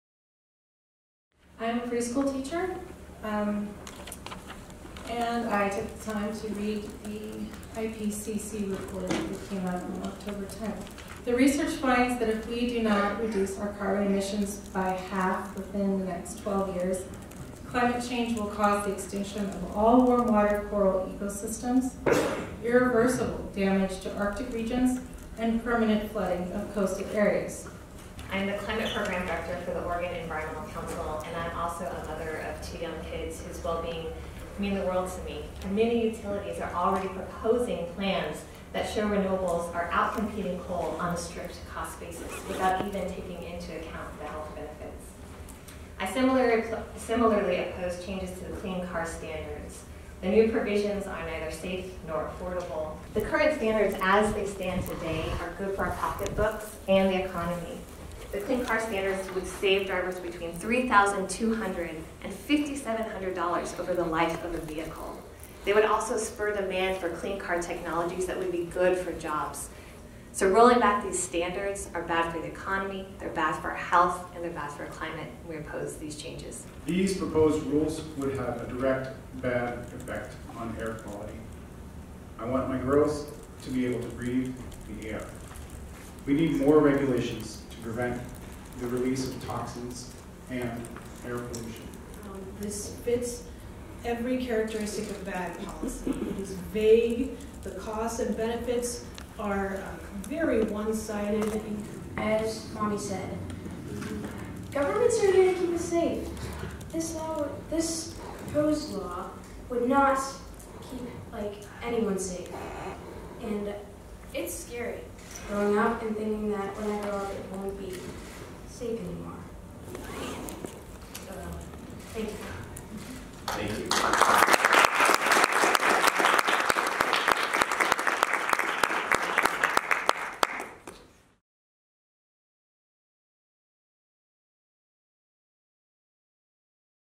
Teacher, family, OEC share concerns for EPA rollbacks of standards
Dozens of concerned Oregonians left messages of opposition to proposals that would reverse environmental standards that protect our climate from greenhouse gases and fossil fuel pollution during a public gathering in Portland. The hearing, organized by the state Department of Environmental Quality and Multnomah County, included a court reporter so that residents, families, doctors, teachers and others could formally speak their concerns.

Others left written statements with officials who ensured they would reach the appropriate federal agencies in time for deadlines. Check out our videos for various moments of testimony and read OEC Climate Program Director Jana Gastellum's message. Oregon Environmental Council strongly opposes these reversals, and asks that you do too.

Deadlines for public comment are rapidly approaching at the end of the month.